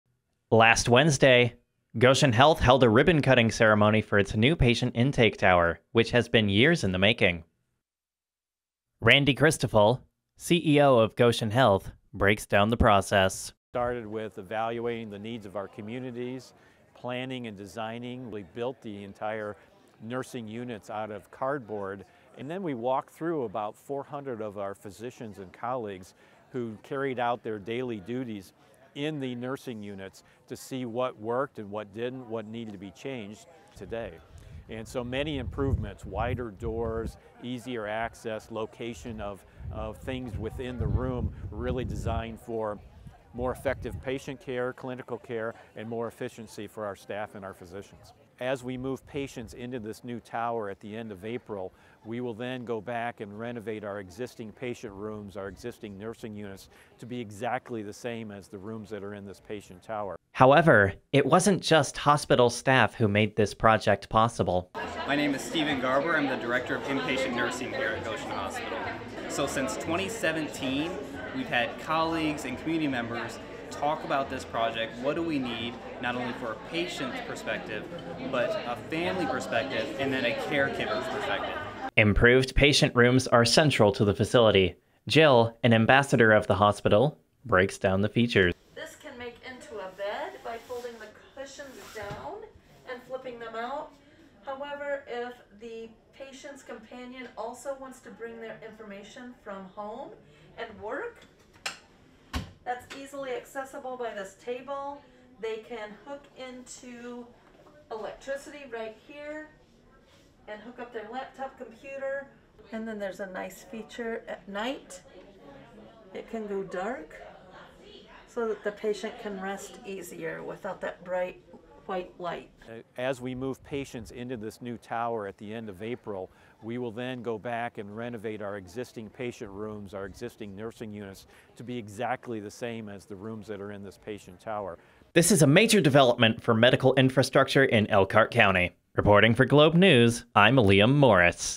Goshen Health Opens New Patient Tower
Last Wednesday, Goshen Health opened a new patient tower at their hospital.

Randy Christophel, CEO of Goshen Health, breaks down the design process, which entailed building a modular scale model of the tower layout from cardboard. Stephen Garber, inpatient nursing director, talks about community involvement in the project. Finally, ambassador Jill Gariss gives a tour of a new patient room.

Christophel says that once patients are moved into the new tower in April, renovations on the rest of the hospital will begin. The patient rooms will look just like these new ones, which are equipped with a work/sleep station for a family member, bathrooms with showers, and information monitors with dark mode.

This is a major development in public health infrastructure in Elkhart County and the Michiana area at large.